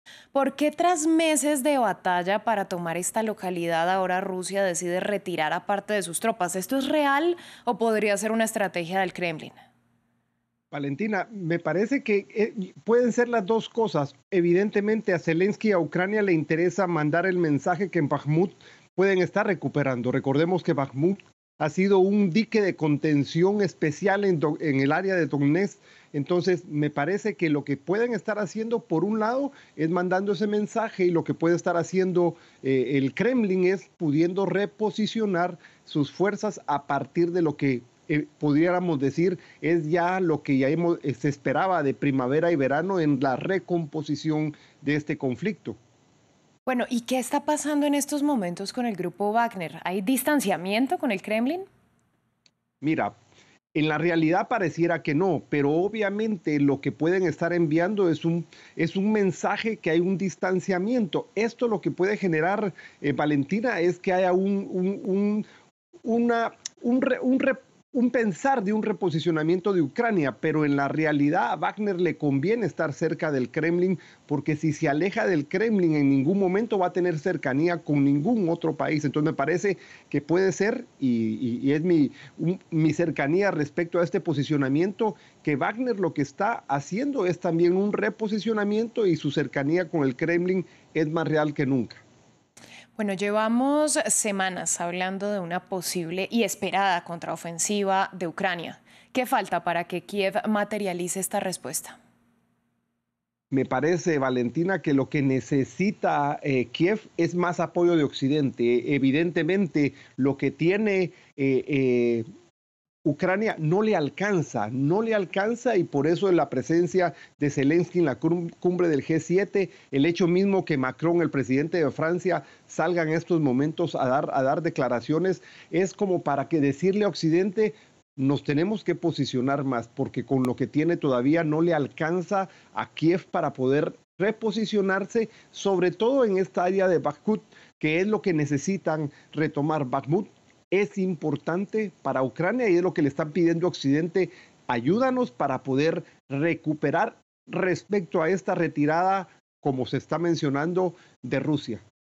Guillermo Pacheco: 'Podría haber un distanciamiento entre el grupo Wagner y el Kremlin'
Desde el 25 de mayo, las tropas del grupo de mercenarios Wagner declararon el inicio de un repliegue territorial en la ciudad de Bakhmut, ubicada al este del país. Con este movimiento militar, tropas rusas comenzaron a ocupar los espacios conquistados por el grupo que es considerado como terrorista. Guillermo Pacheco, director de asuntos internacionales del Security College US, habló sobre la situación actual en esta ciudad en France 24.